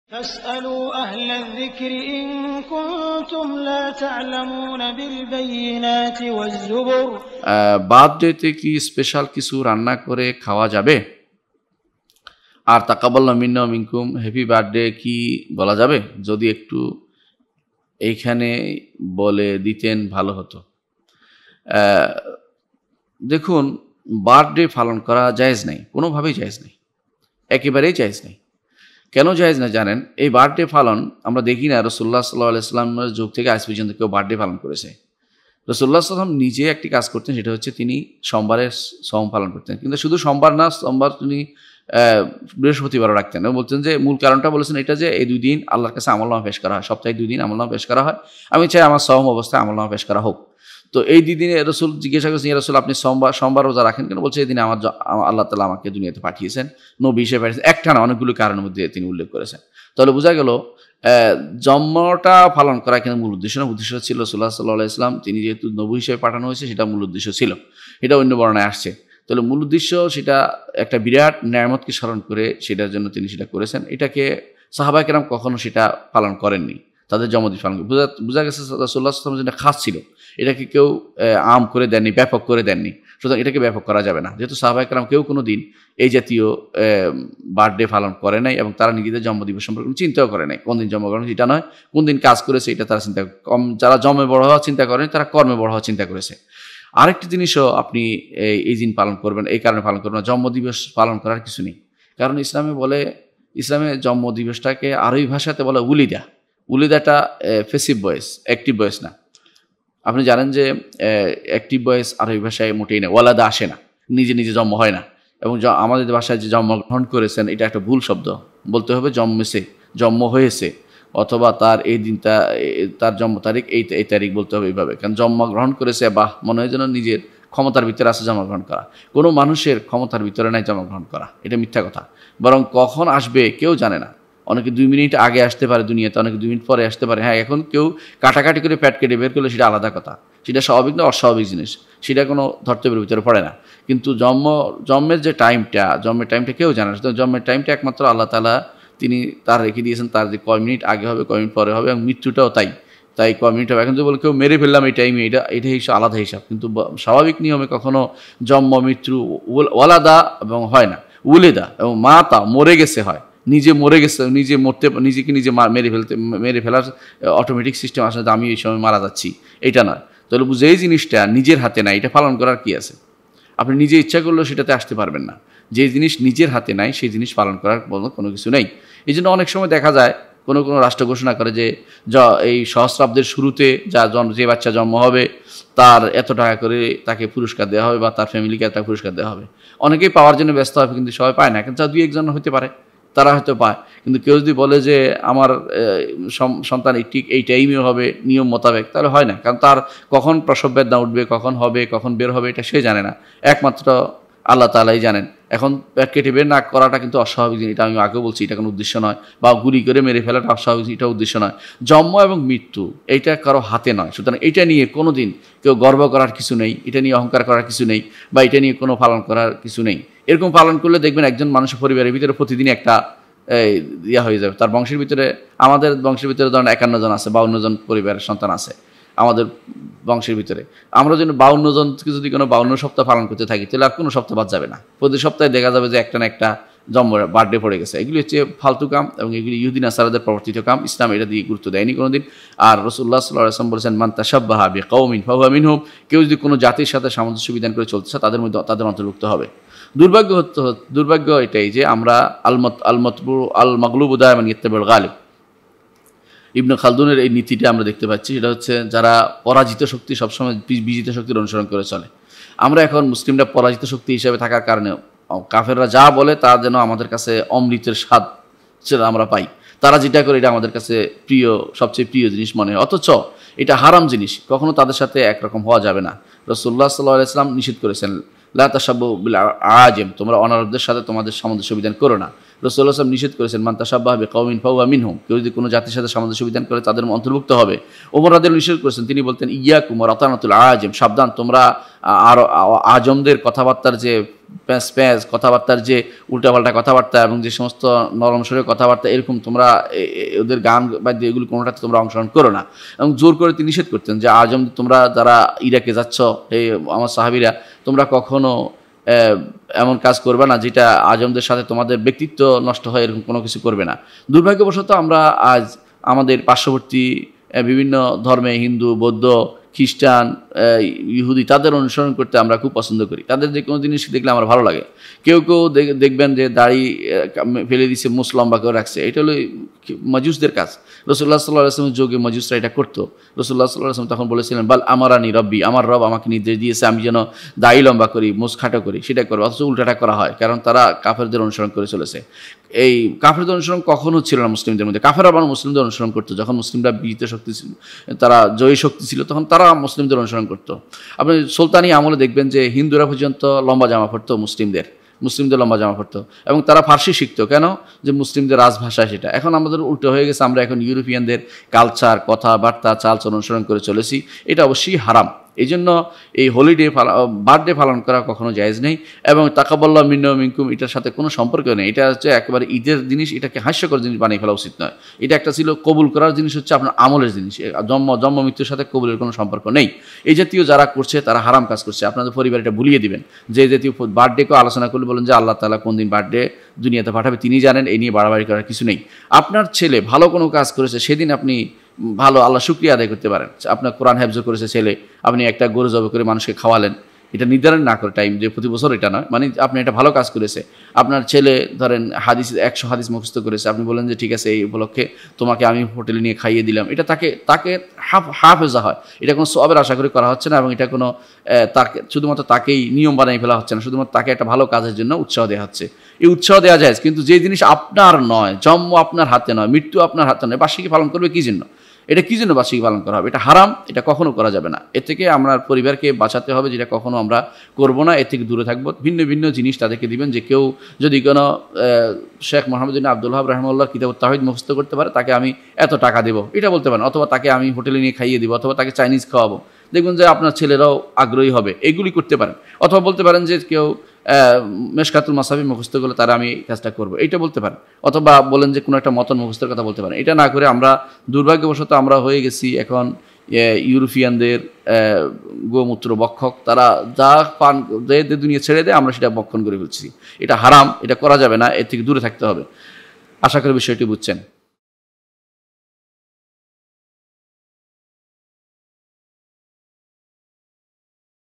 প্রশ্ন : জন্মদিনে কিছু রান্না করে খাওয়া যাবে? শাইখ প্রফেসর ড. আবু বকর মুহাম্মাদ যাকারিয়া
প্রশ্ন : জন্মদিনে কিছু রান্না করে খাওয়া যাবে?
শাইখ প্রফেসর ড. আবু বকর মুহাম্মাদ যাকারিয়া